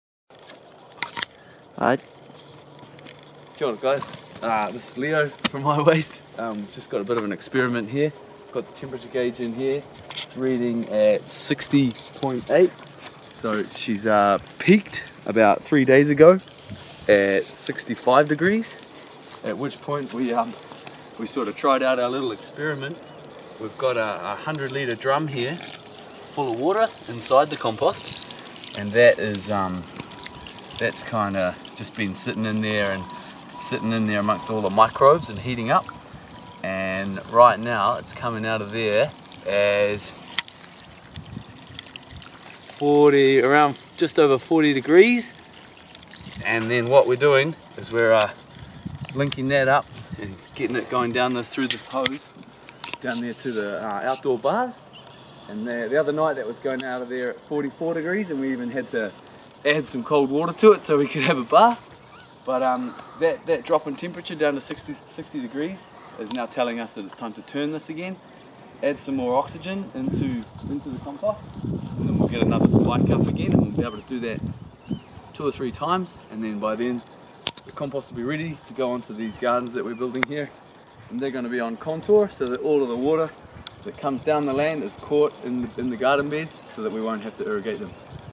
Hot water from compost - free biological energy
We built the 3metre cubed pile around a 100 litre drum rescued from the tip. We layered in the funky food waste from our compost collection with a variation of different materials, all contributing different elemental or structural benefits. These include: Hardwood sawdust, scrub and dead grass, horse manure, cow manure, leaf litter, clay, cardboard, old compost, new compost, fungal compost, lime and paramagnetic rock dust.

The result: Enough water at 44°C to heat two baths to be enjoyed by four people - on a freezing night!